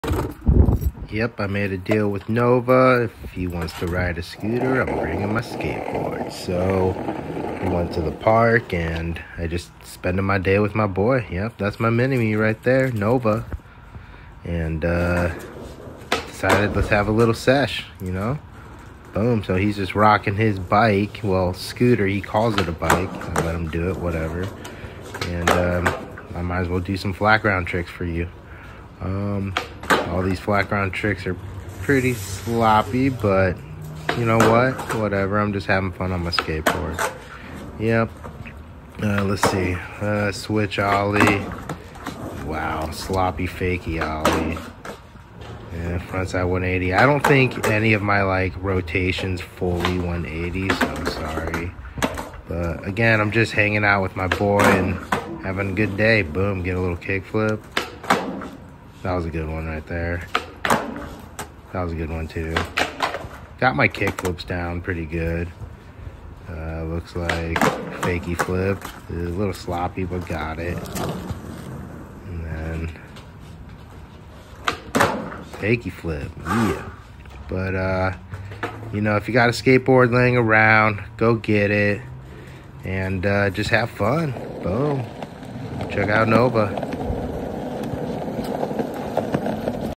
Flat ground skate sesh.
Flat ground sesh at the park with my son Nova.